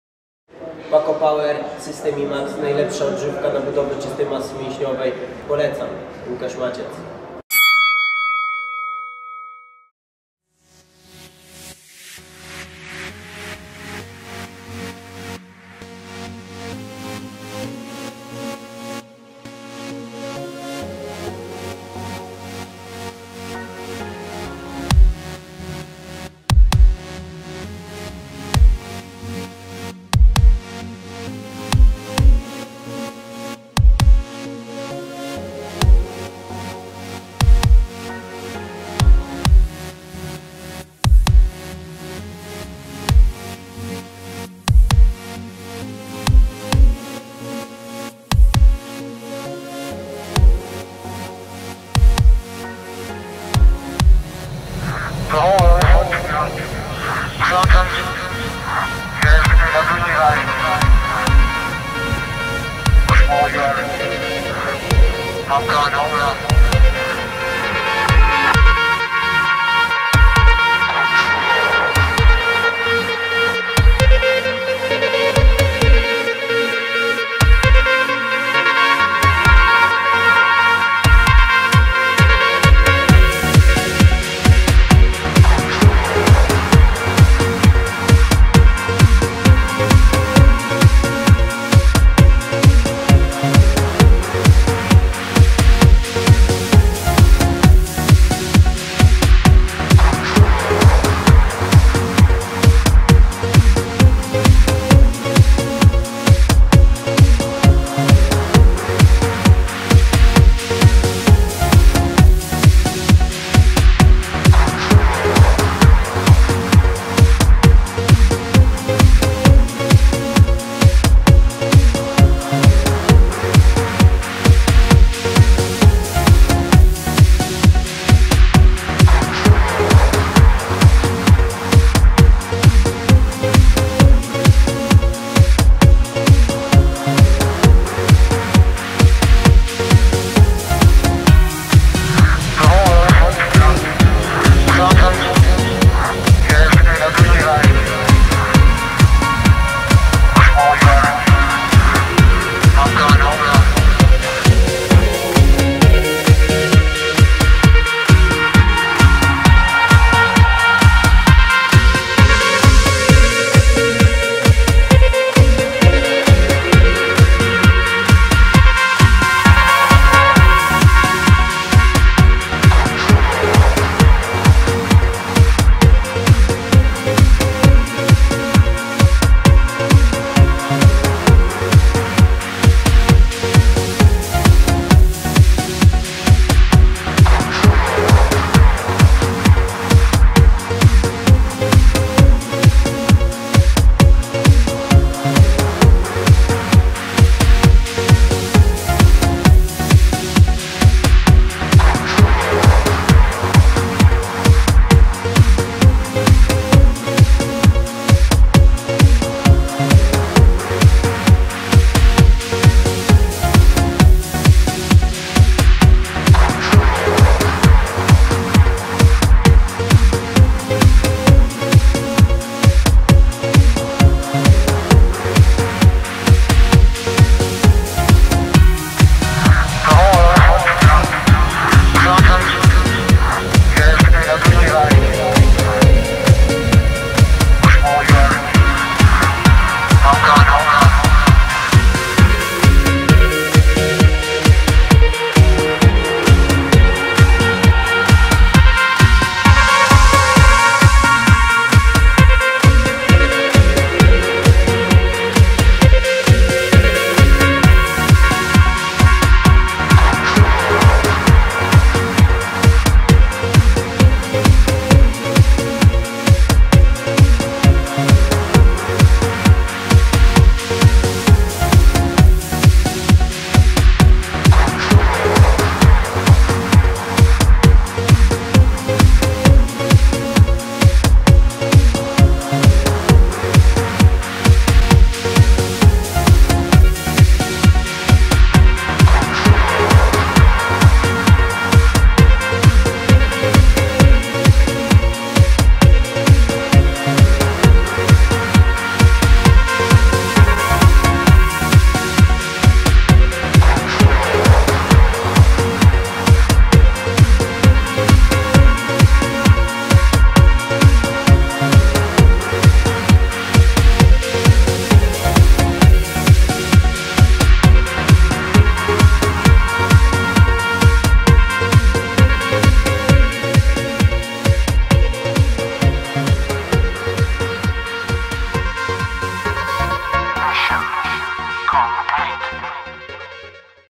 Karolina Bolek w pracy, oraz na treningu!
Zapraszamy do obejrzenia materiału z treningu, oraz z pracy Pani Karoliny Bolek, z obsługi KS PACO przy ul. Zana 72